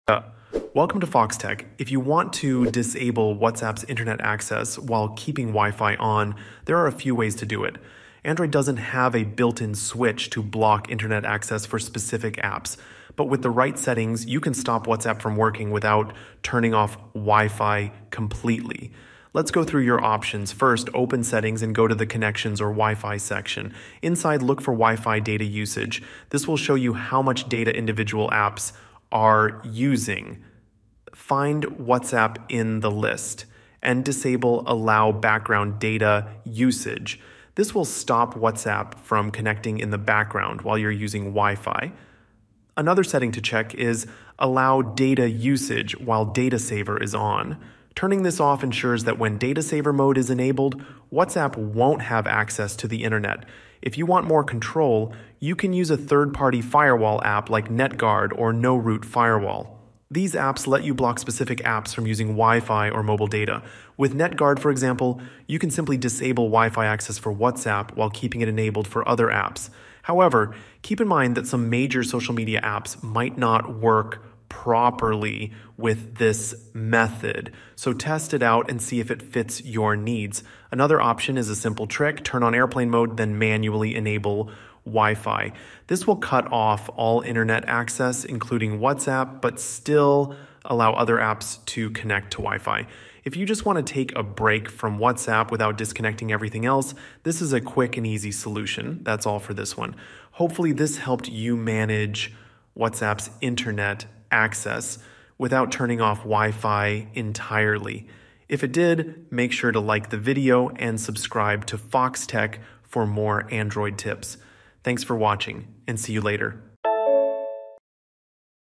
How to Turn Off WhatsApp when Wifi Is On in Android (explained)
In this video, I discuss how to prevent whatsapp from accessing wifi or cellular data and how this can be manually adjusted, at least, to a degree. All the important information mentioned in the video.